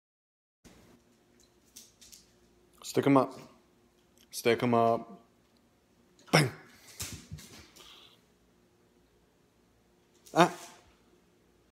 Stella Breaks Character
Stella plays dead, broke character for a minute lol
-Steven Blakeley